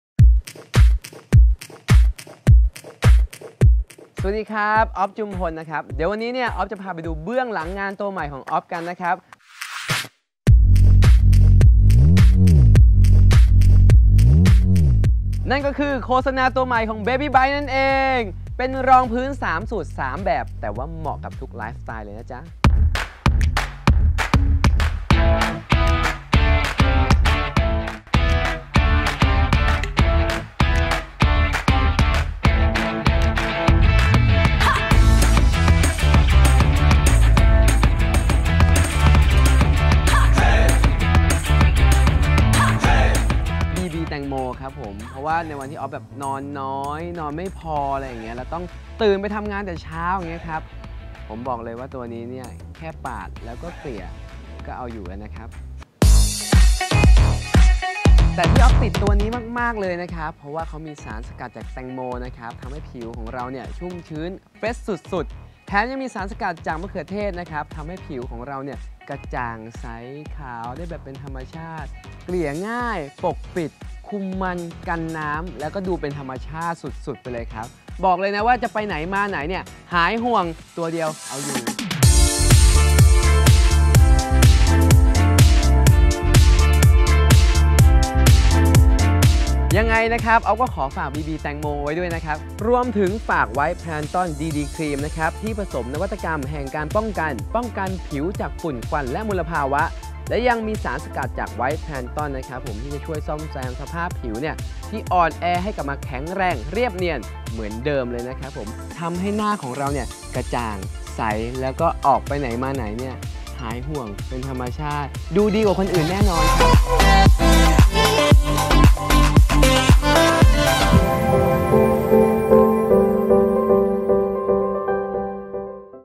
OFF x BBWATERMELON & WHITE PLANKTON DD : Behind the Scene
อั๊ยยยย ป่าปี้หน้าใสงานดีเวอร์ ตามมาดูกันค่าา🌟ว่าไอเท็มตัวไหนกันน้า..ที่เป็นตัวช่วยให้หนุ่มออฟ👨💚หน้าใสเวอร์ได้เบอร์นี้
ถึงจะอดนอนหน้าโทรม แค่ปาดก็เอาอยู่แล้ว หรือจะอัพลุคใสๆ เตรียมผิวสู้มลภาวะก็ไม่หวั่น หน้ากระจ่างใสได้ตลอดวัน คือดีต่อใจ ป่าปี้เค้าฝากไว้ พลาดไม่ได้น้าาา 😍💚💚💚